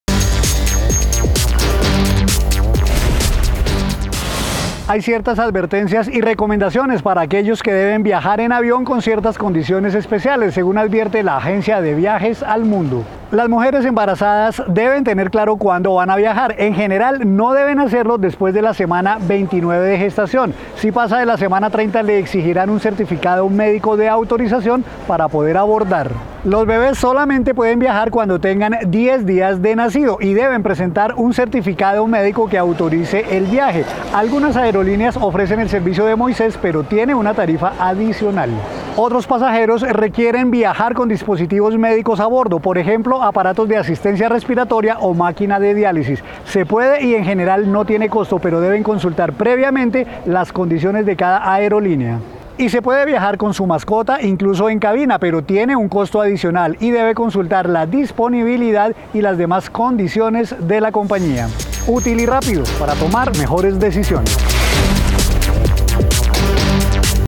Para viajar en avión debe cumplir unos requisitos si pertenece a este grupo de personas
Embarazadas, bebés, pacientes y mascotas deben tener en cuenta lo que exigen las aerolíneas para su abordaje.